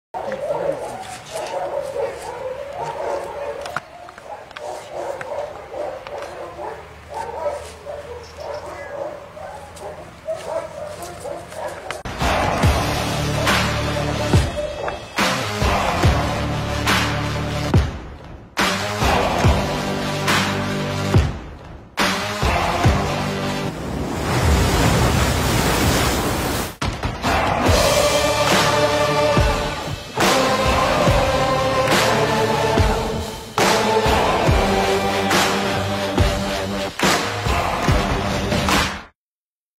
Caucaso vs Gatto
Ottenere questo risultato con due "bolidi" di Caucaso di cui 100 kg. Il maschio e 70 kg. la femmina non è facile e soprattutto : non da tutti e non per tutti ! Il messaggio è solo quello di comunicare che grazie all'addestramento pure i cani più difficili possono essere recuperati , questo fa si che i proprietari se li possano godere...evitando abbandoni e/o soppressioni! Grazie all'amico Emilio Cau per avermi aiutato nel mettere in essere il video .